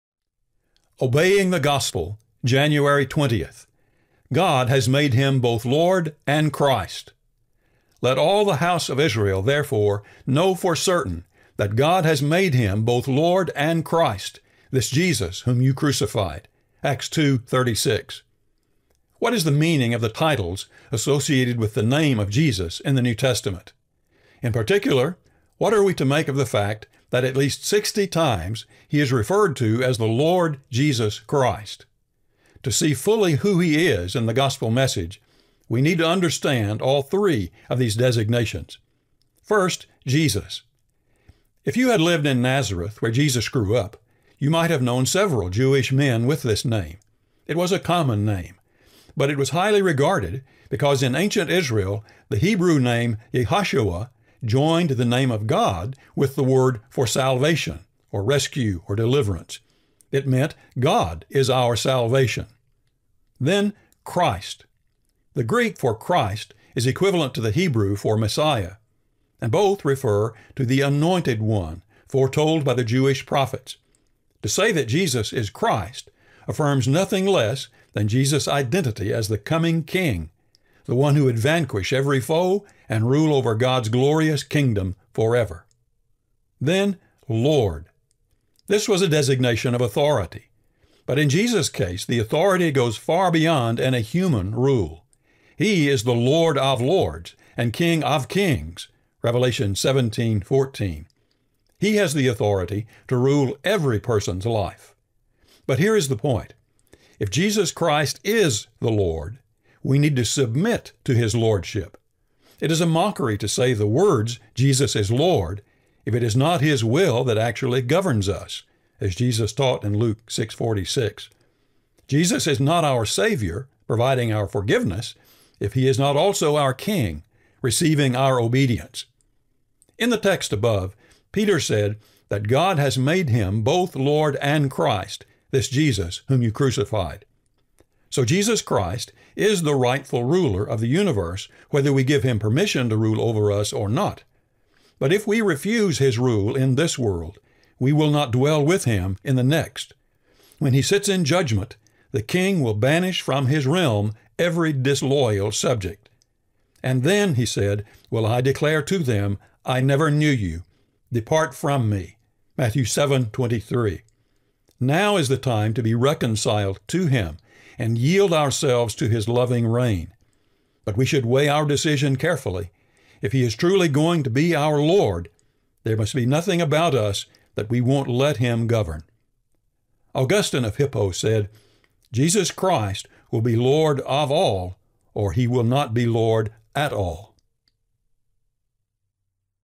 God Has Made Him Both Lord and Christ (January 20)
"Let all the house of Israel therefore know for certain that God has made him both Lord and Christ, this Jesus whom you crucified" (Acts 2:36).

WHAT IS THE MEANING OF THE TITLES ASSOCIATED WITH THE NAME OF JESUS IN THE NEW TESTAMENT? In particular, what are we to make of the fact that at least sixty times He is referred to as the “Lord Jesus Christ”? To see fully who He is in the gospel message, we need to understand all three of these designations.

JESUS. If you had lived in Nazareth where Jesus grew up, you might have known several Jewish men with this name. It was a common name, but it was highly regarded because in ancient Israel the Hebrew name Yehoshua joined the name of God with the word for salvation (rescue, deliverance). It meant God is our Salvation.

CHRIST. The Greek for Christ is equivalent to the Hebrew for Messiah, and both refer to the “anointed one” foretold by the Jewish prophets. To say that Jesus is “Christ” affirms nothing less than Jesus’ identity as the coming King, the One who would vanquish every foe and rule over God’s glorious kingdom forever.

LORD. This was a designation of authority, but in Jesus’ case, the authority goes far beyond any human rule. He is the “Lord of lords and King of kings” (Revelation 17:14). He has the authority to rule every person’s life. But here is the point: if Jesus Christ is the Lord, we need to submit to His lordship. It is a mockery to say the words “Jesus is Lord” if it is not His will that actually governs us (Luke 6:46). Jesus is not our Savior (providing our forgiveness) if He is not also our King (receiving our obedience).

In the text above, Peter said that “God has made him both Lord and Christ, this Jesus whom you crucified.” So Jesus Christ is the rightful Ruler of the universe whether we give Him permission to rule over us or not. But if we refuse His rule in this world, we will not dwell with Him in the next. When He sits in judgment, the King will banish from His realm every disloyal subject — “And then will I declare to them, ‘I never knew you; depart from me’” (Matthew 7:23). Now is the time to be reconciled to Him and yield ourselves to His loving reign. But we should weigh our decision carefully. If He is truly going to be our Lord, there must be nothing about us that we won’t let Him govern.

"Jesus Christ will be Lord of all or he will not be Lord at all" (Augustine of Hippo).